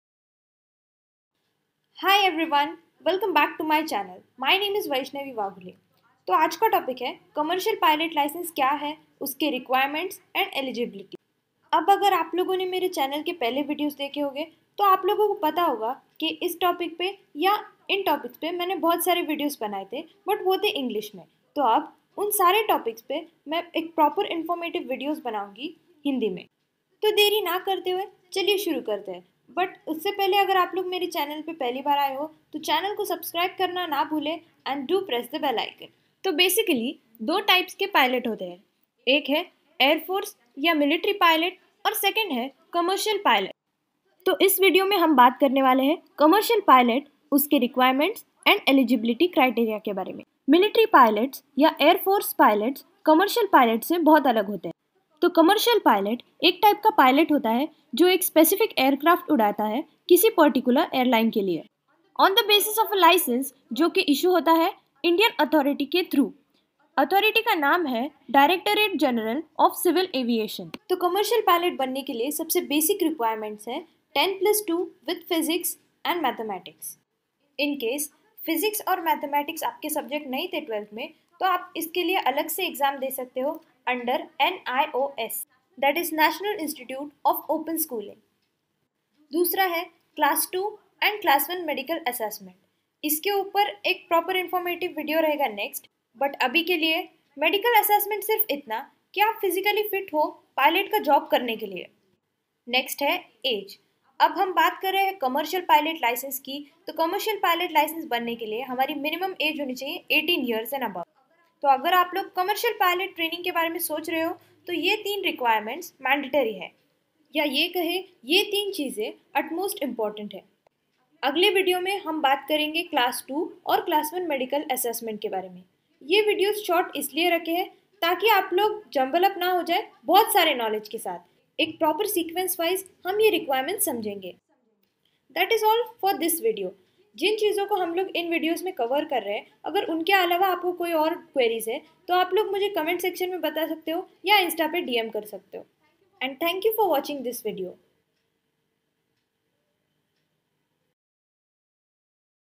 CPL(COMMERCIAL PILOT KAISE BANE) part 1
HELLO ASPIRANTS THIS  IS THE FIRST PART ON HOW TO BECOME A PILOT IN HINDI.